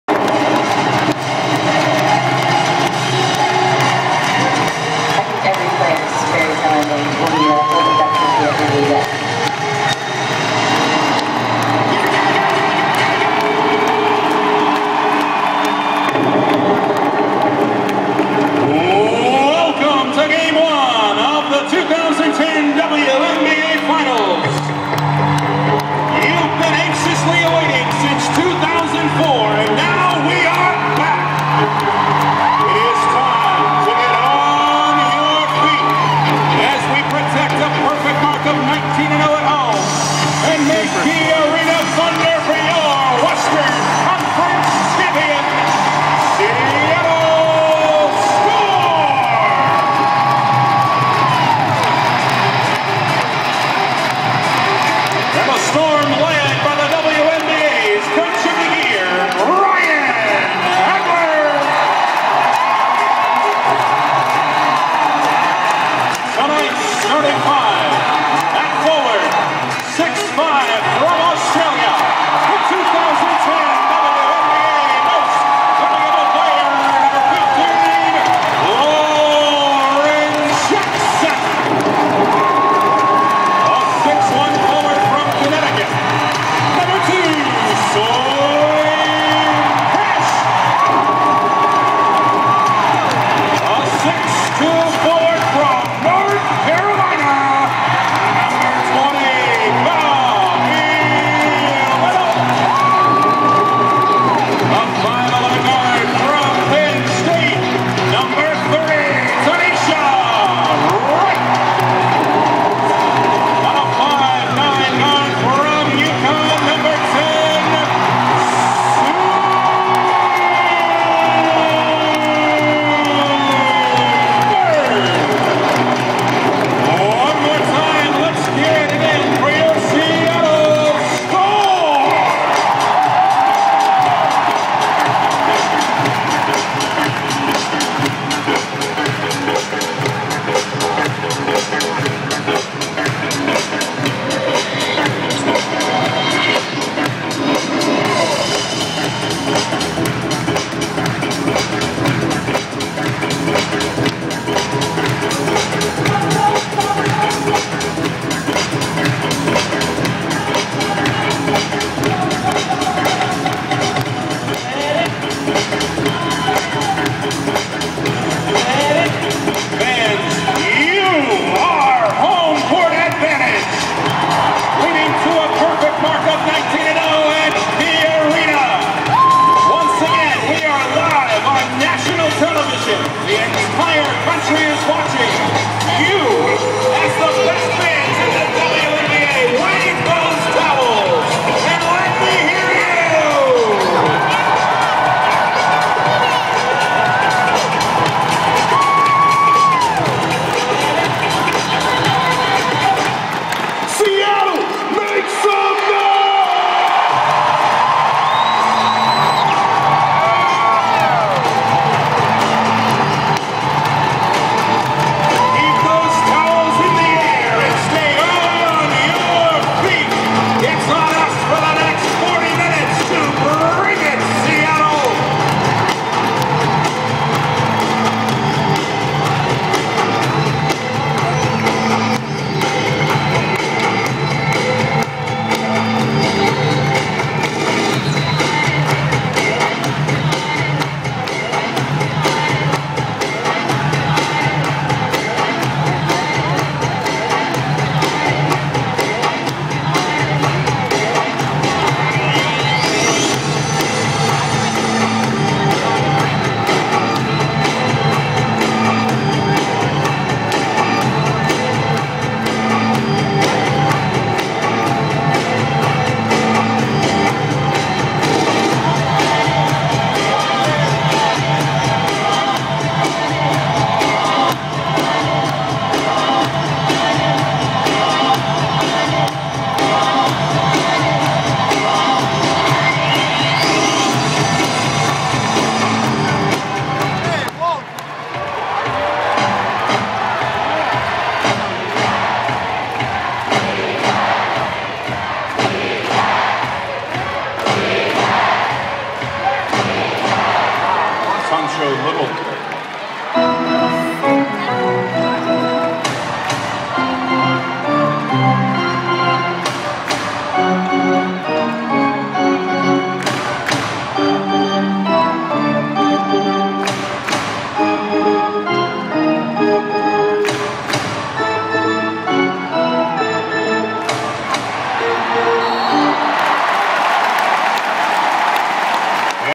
2010 WNBA Finals Seattle Storm Player Introductions
The Seattle Storm player introductions at game 1 of the 2010 WNBA Finals at KeyArena in Seattle, Washington.